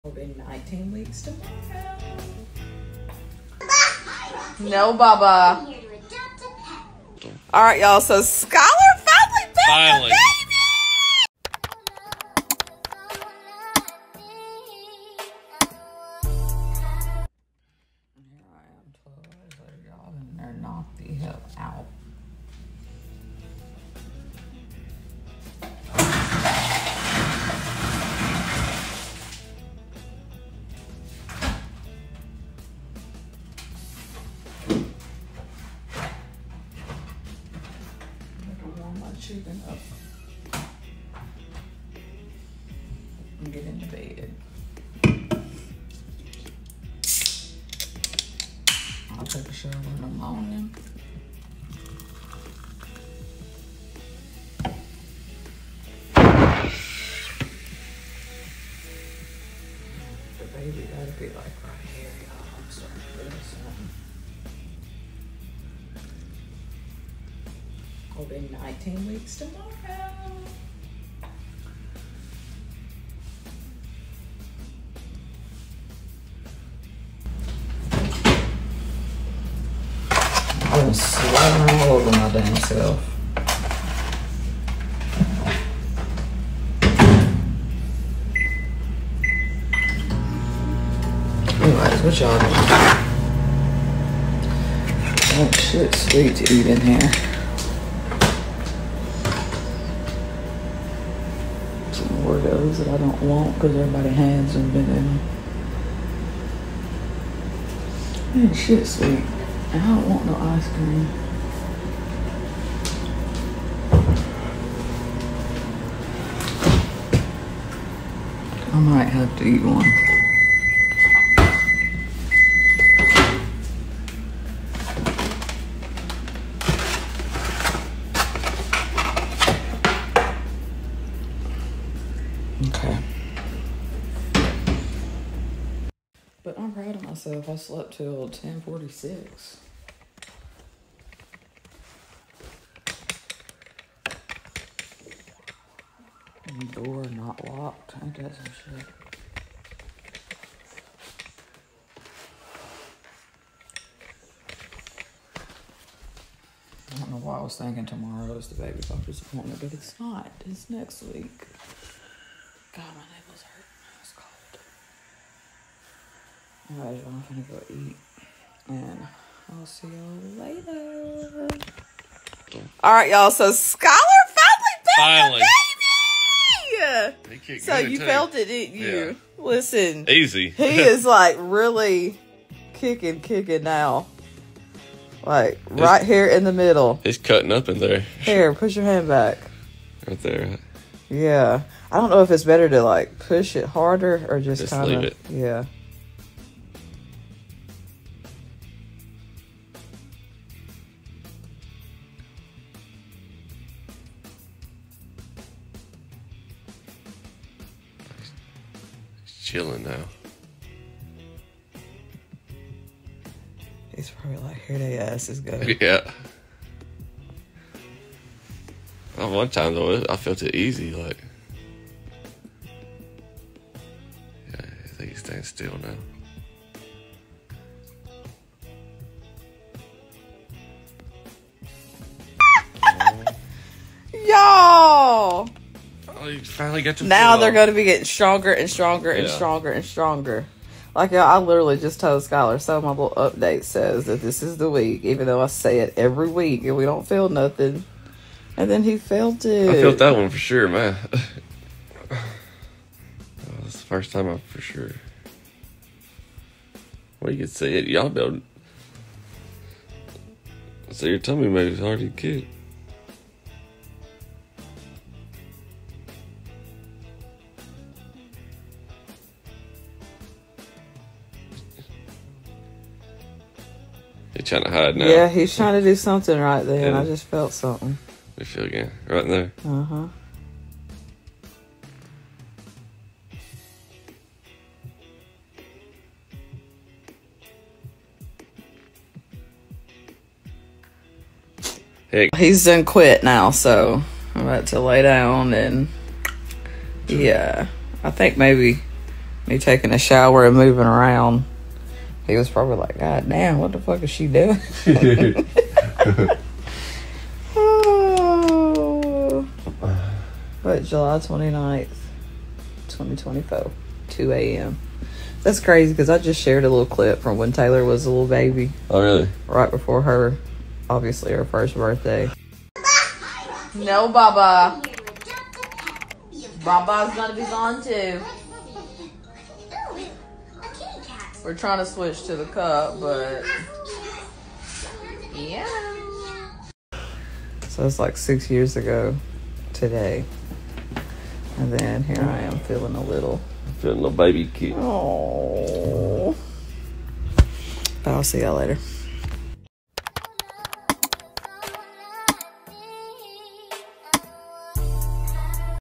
19 Weeks Today | 6 Years Ago Today | Dad Felt Baby Kick For The First Time!
My sleeping schedule is soooo off this pregnancy lol so here I am in the kitchen getting a snack at midnight after sleeping all day nearly.

Also here’s a clip that popped up in my memories of my baby girl Tayler, ohhh my baby is about to be six years old, bittersweet moments!!

Then I started feeling STRONG movement and I just knew dada would be able to feel it and sure enough he DID!!!

I’m exactly 19 weeks today so the kicks will only be getting stronger and stronger!

Love you guys & more updates to come, hope you enjoy, Love Momma K!! MUAHHH!